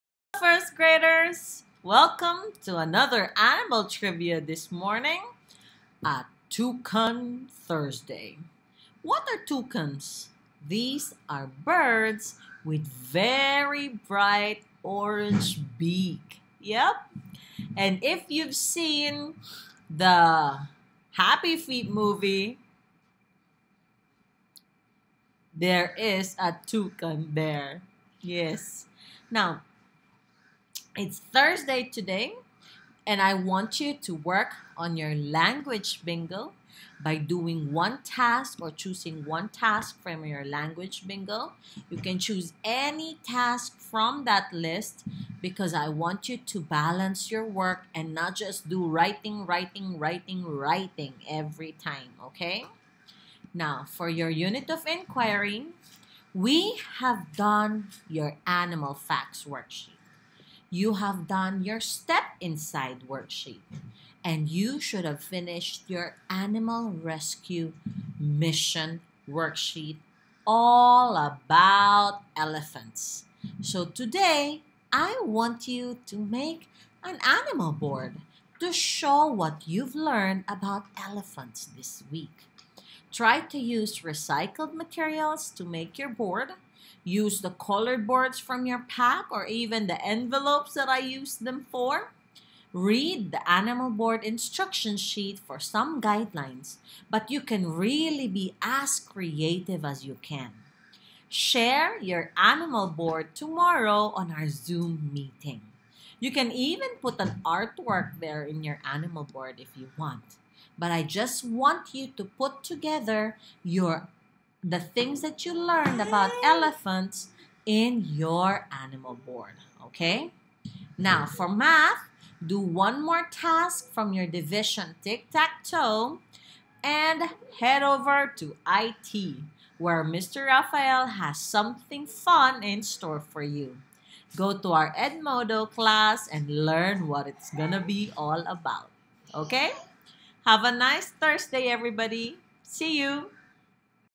Week 7 Day 4 Toucan Thursday Morning Meeting with Mrs G
Learn more about the colourful Toucan from this Toucan Thursday Morning Meeting Animal Trivia! Watch this video to find out what you can do for the day.

Keep doing one task from your Language Bingo to learn more about Language. You can chose to practice your alphabetizing skills, dictionary skills, letter writing skills, or sentence writing skills. It's entirely up to you! Just be sure to balance your work for the week.

Next, for your Unit of Inquiry, make an Animal Board! Show what you learned about the elephant this week. Try to use recycled materials to make your board or use the coloured boards in your pack. Read the Animal Board instruction sheet for some guidelines. Share your Animal Board tomorrow on our Zoom Meeting.

Choose one task from your Division TIC TAC TOE to practice your division. Remember, if you need extra help in learning about division, let me know so we can have a Division Zoom Meeting and practice. Okay?

It's Thursday again today and Mr. Raph has prepared something fun and exciting for you. Head to our Edmodo class to find out more!

Energise your day by dancing with the animals! Watch and move along with these videos.

Have a terrific Toucan Thursday!